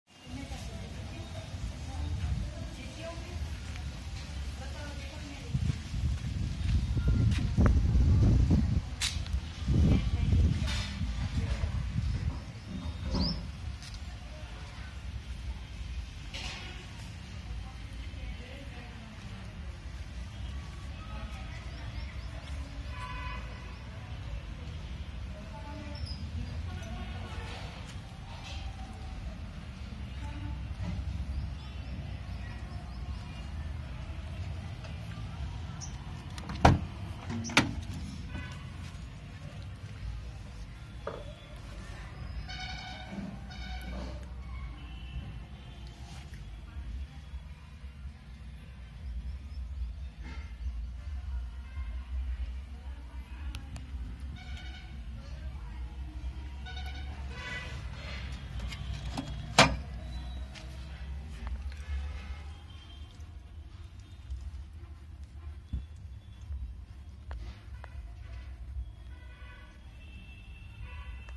New Maruti Suzuki EECO 2017, 5 SEATER AC
The all new Maruti EECO with head rest rear seat and new graphics around it.